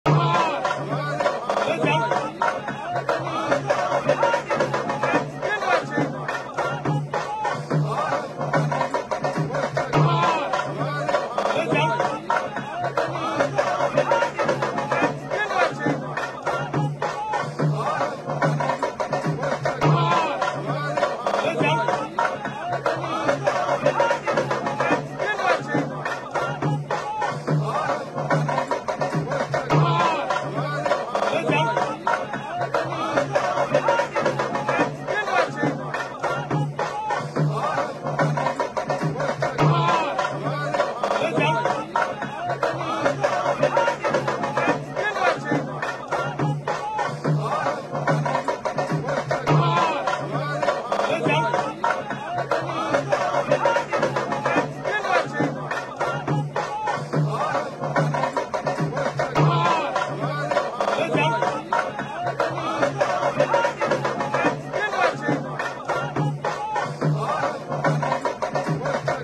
BRS MLA లాస్య నందిత పాడే మోసిన హరీష్ రావ్ | BRS MLA Lasya Nanditha | harish rao | Manasarkar
BRS MLA లాస్య నందిత పాడే మోసిన హరీష్ రావ్ | BRS MLA Lasya Nanditha | harish rao | Manasarkar | Harish Rao Tribute to Lasya Nanditha